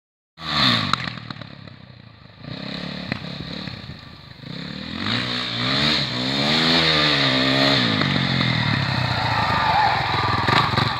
SUZUKI DRZ 400E
Pierwsza jazda syna Michała po przesiadce z MRF 120 na DRZ 400E
(14 lat ). Chyba się dogadują :)